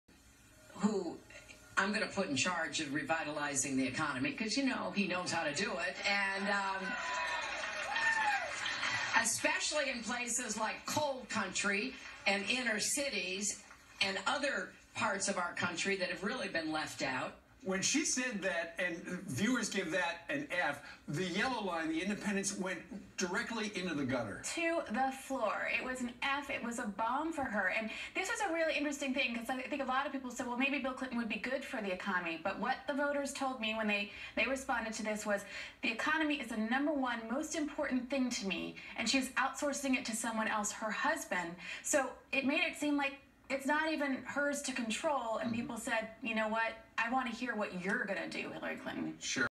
Hillary Wants Bill to Fix Obama Economy
Hillary Wants Bill Clinton to Fix Obama Economy.  How do viewers react to Hillary Clinton's promise to put the former president in charge of economic issues?  Not a good idea.  The yellow line represents Independents, the red Republicans, the blue Democrats.

For years we've tested concepts with viewers using this type of research.  It's proven valuable.  It helps avoid mistakes.  In this case viewers are looking for an outsider to shake things up.  Many don't see Hillary Clinton and Bill as outsiders.